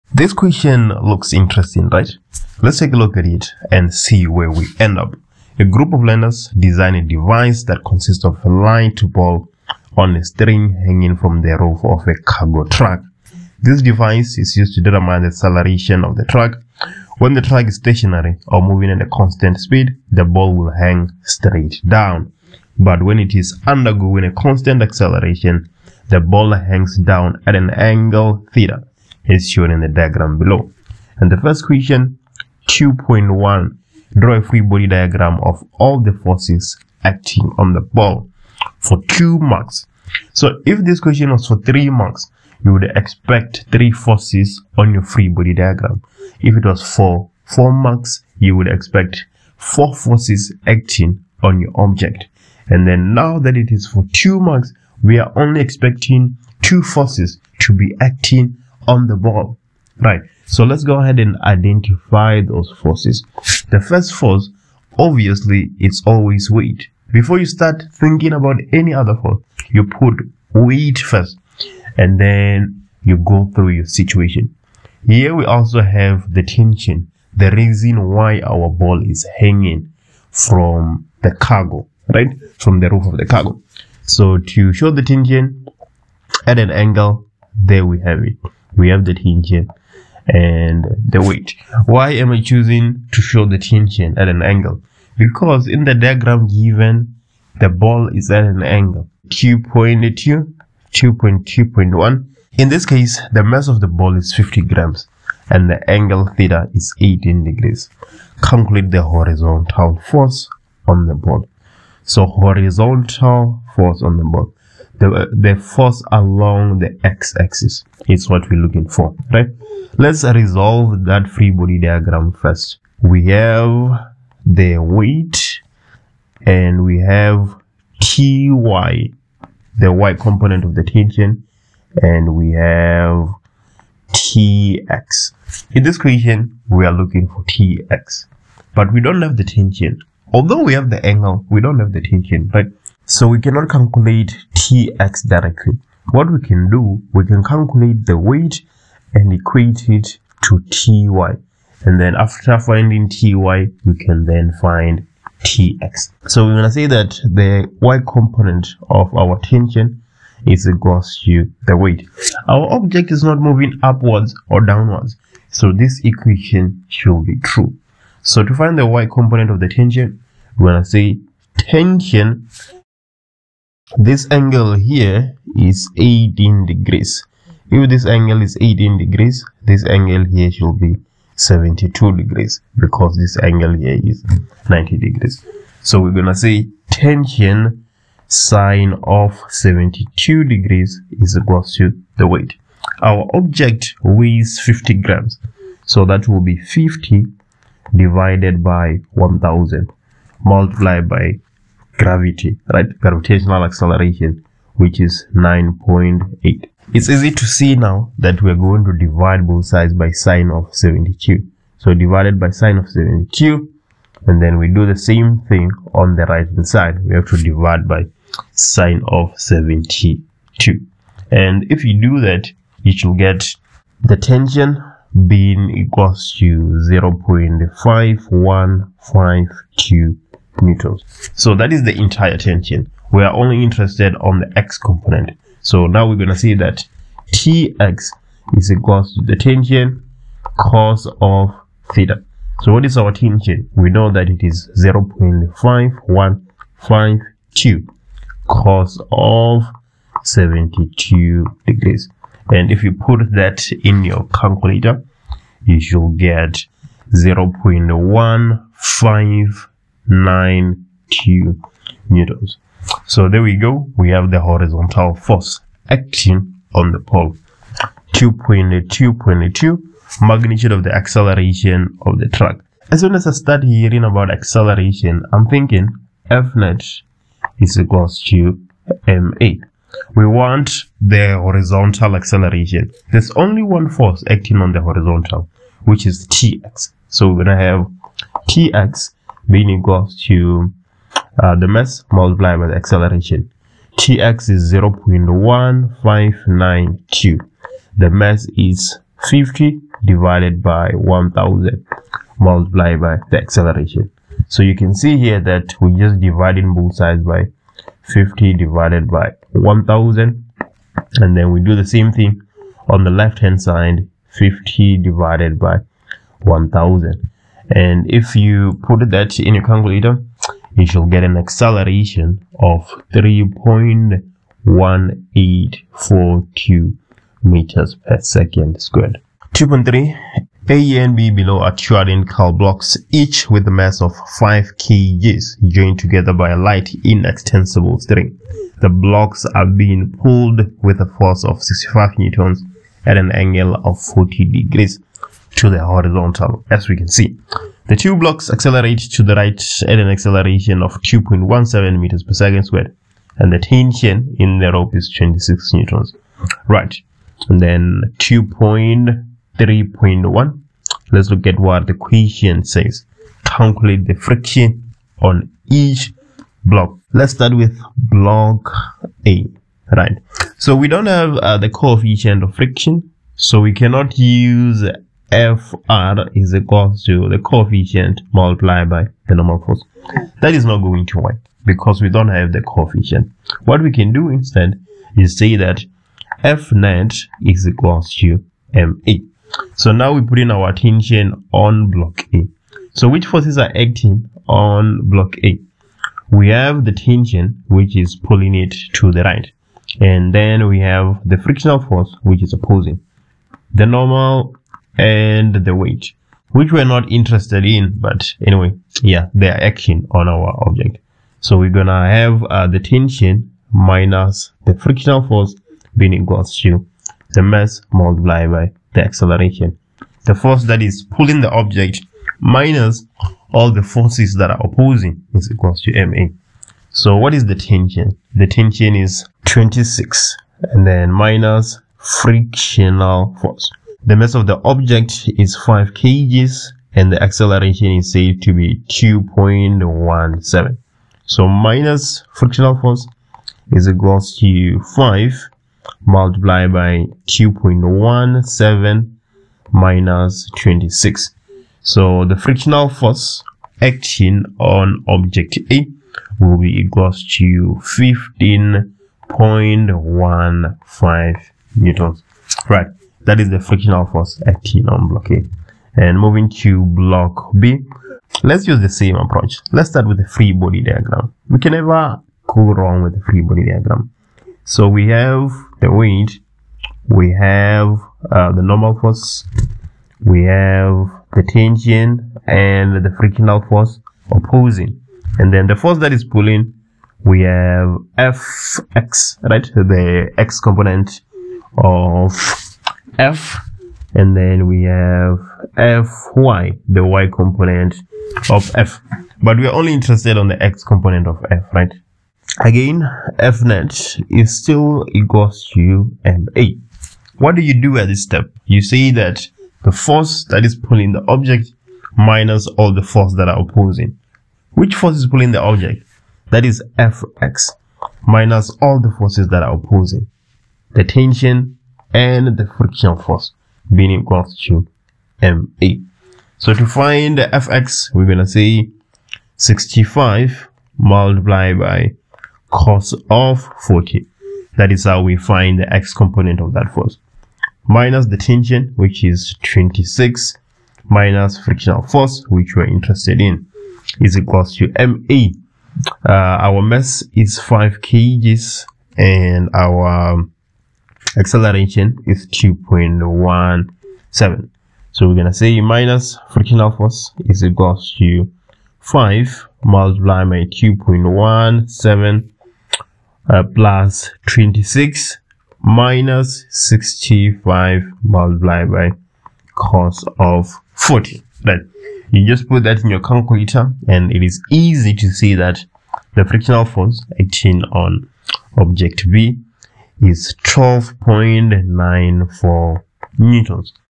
Newton’s Laws of Motion Grade 12 Physics Prelim Gauteng Exam 2022
Question 2 Grade 12 Physical Science September 2022